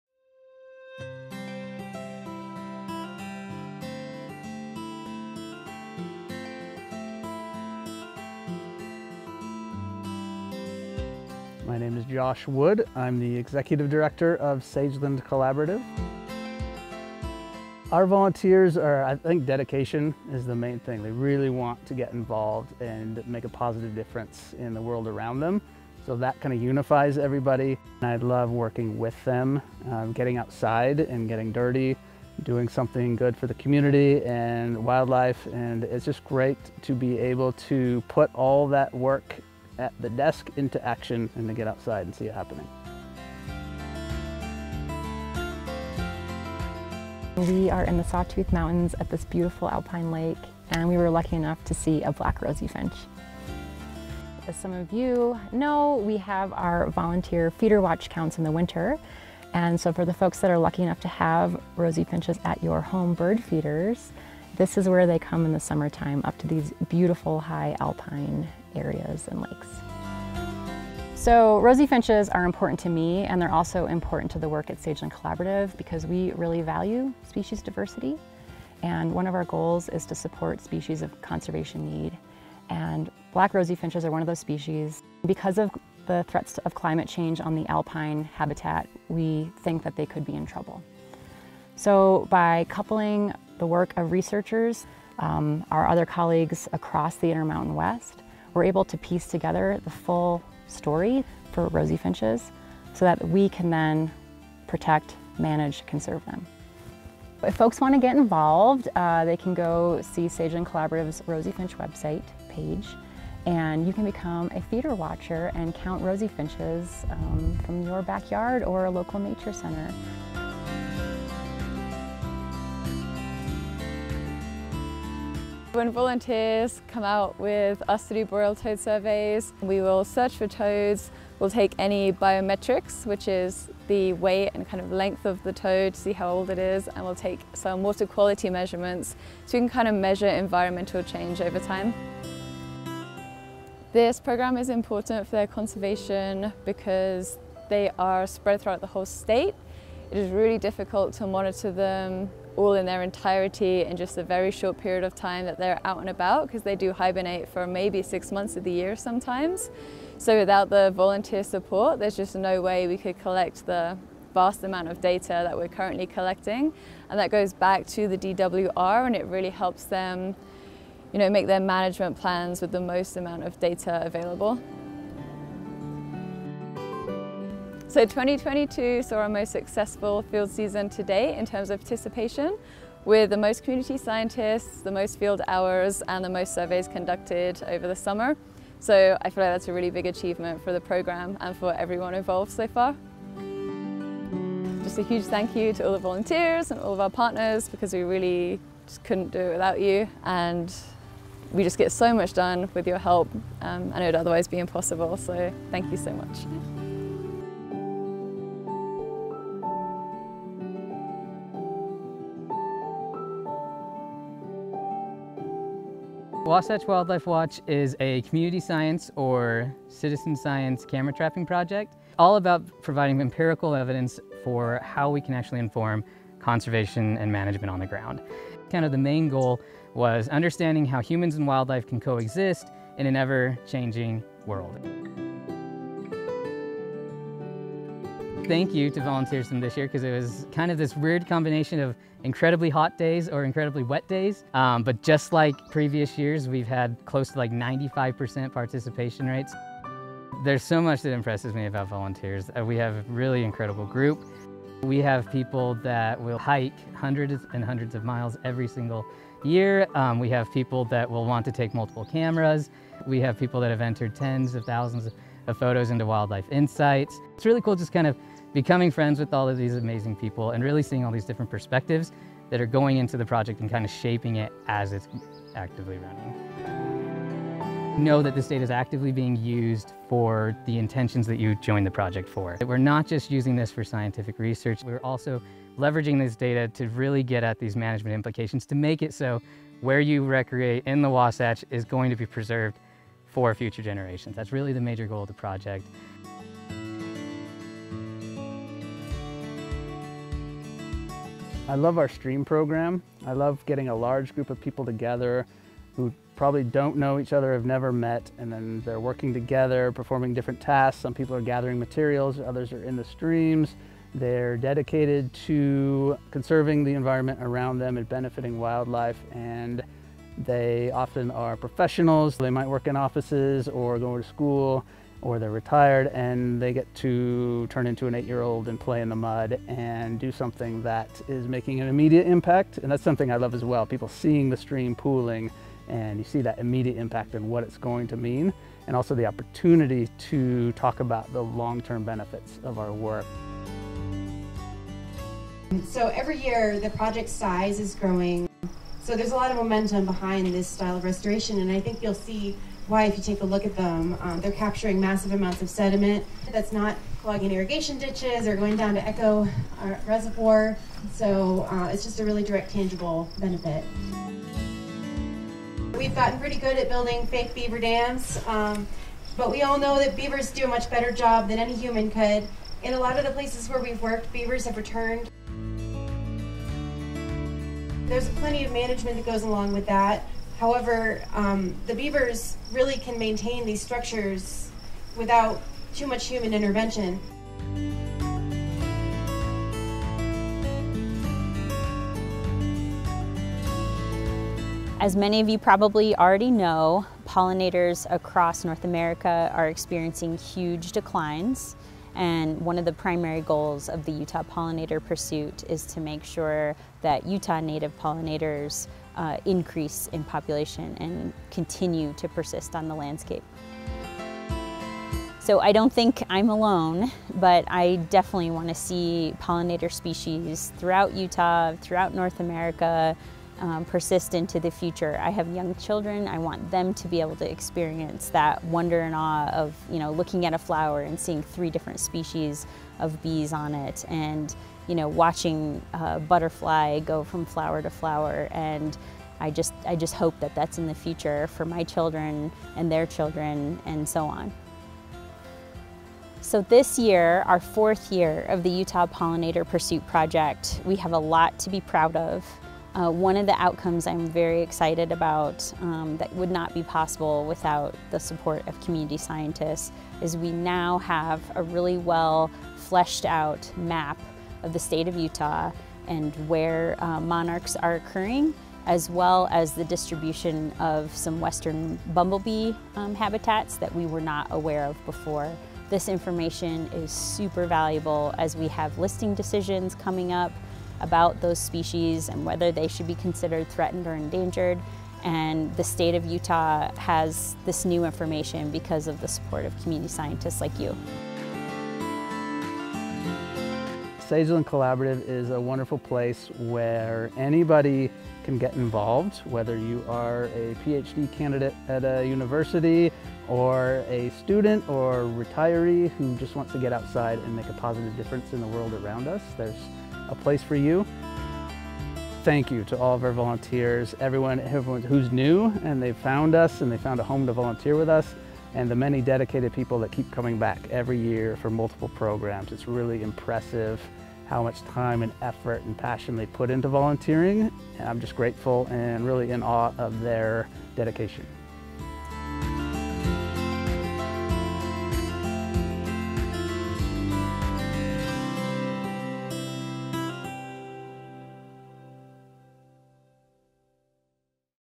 The Best Year Yet for Wildlife Conservation: Thank You 2022 Volunteers!
Learn more about science-based wildlife conservation in Utah and the entire West. Our conservation project leads describe major impacts of the project in 2022. Thank you to our amazing community scientists and volunteers for making this work possible!